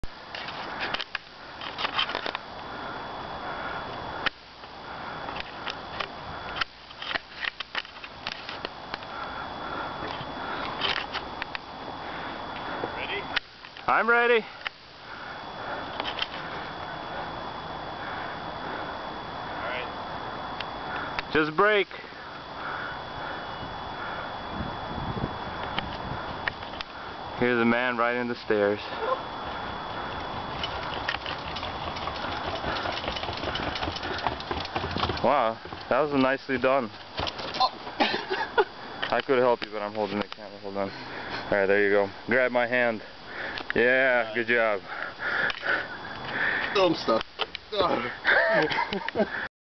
Riding down stairs in Cheesequake Park, NJ
Decided to try riding down some stairs in Cheesequake Park, NJ.  Not difficult, but there wasn't much room on either side.  Was a little tricky at the end.